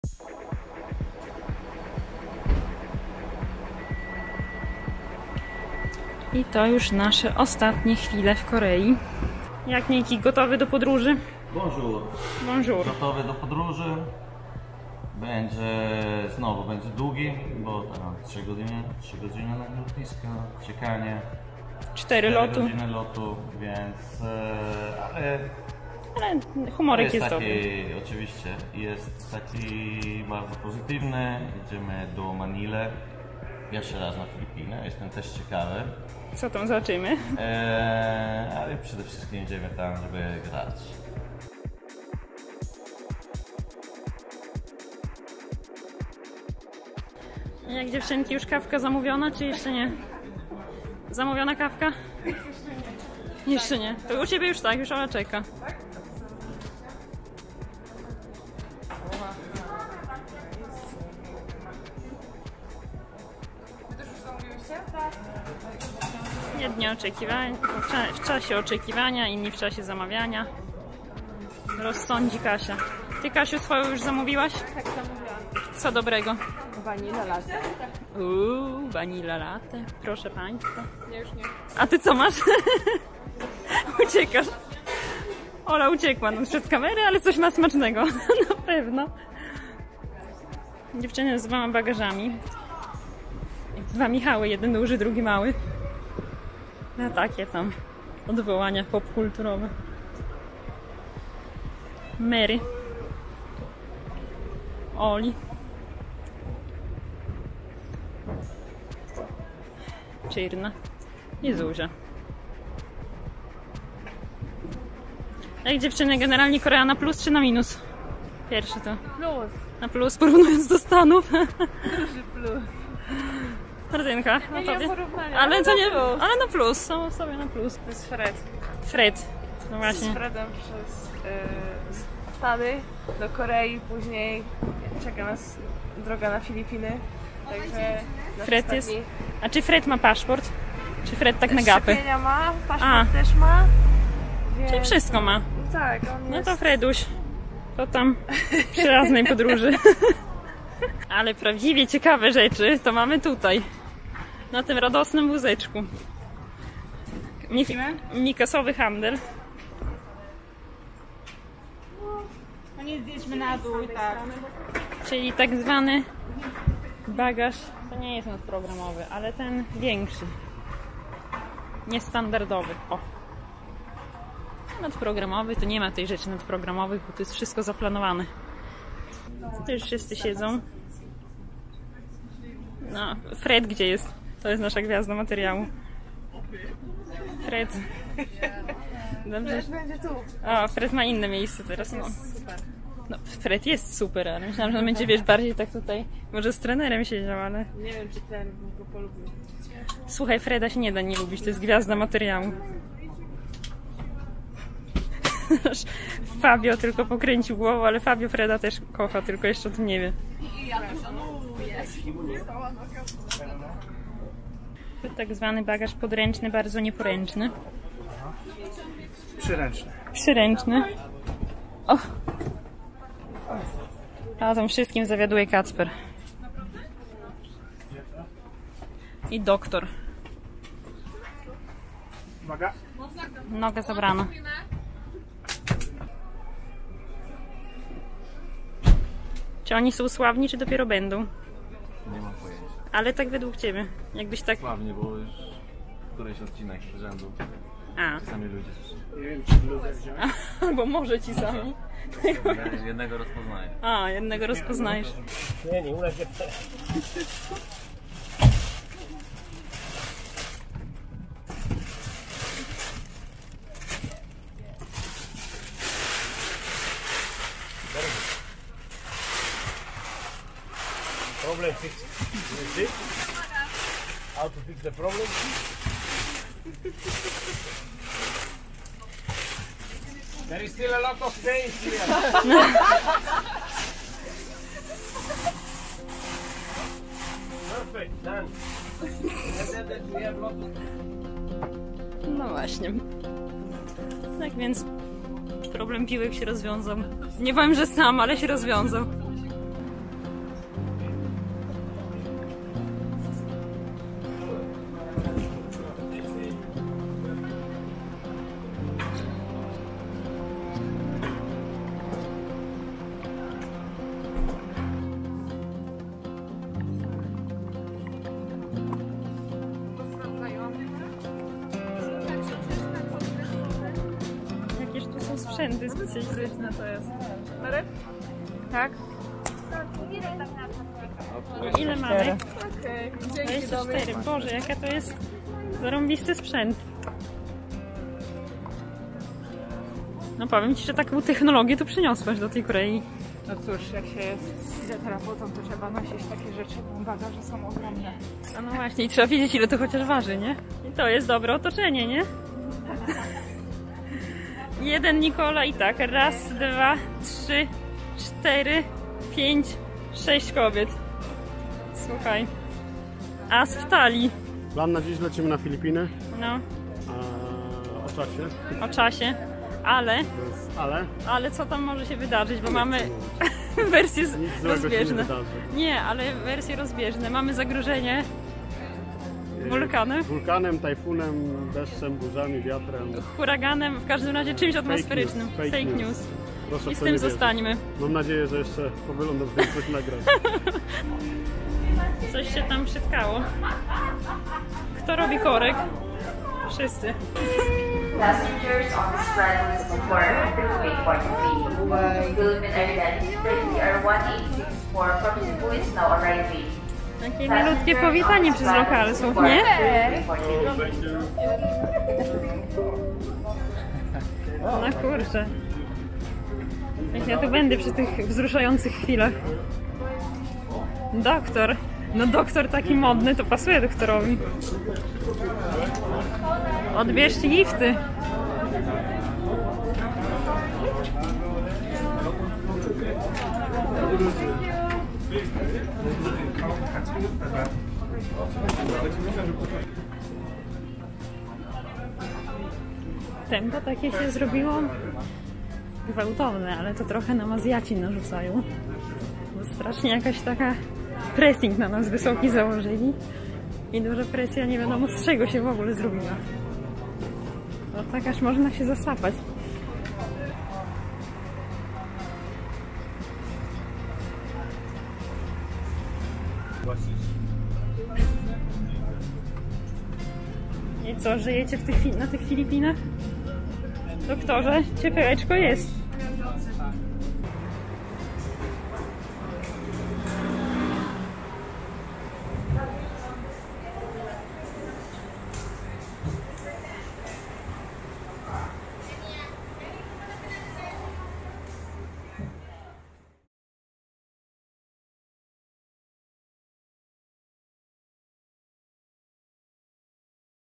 Vlog&Play: Fred, wulkan i piłki
Reprezentacja Polski dotarła na Filipiny, gdzie rozegra kolejne mecze Siatkarskiej Ligi Narodów.